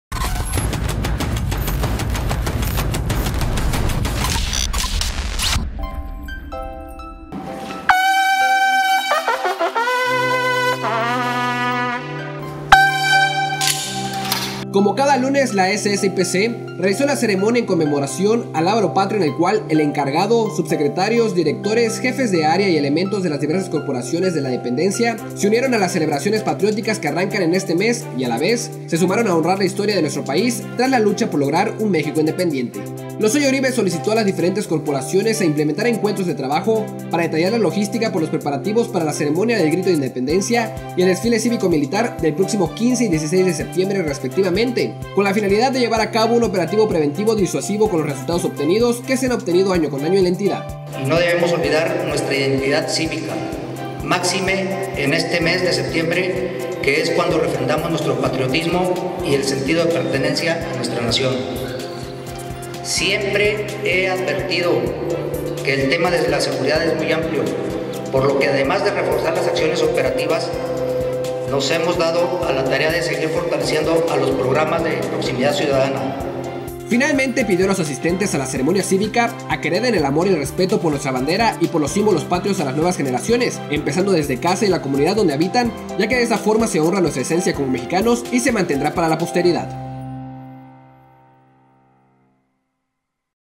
Lozoya Uribe exhorta a trabajar en la logística para las próximas celebraciones de septiembre
Lozoya Uribe exhorta a los cuerpos de seguridad a trabajar en la logística para las próximas celebraciones de septiembre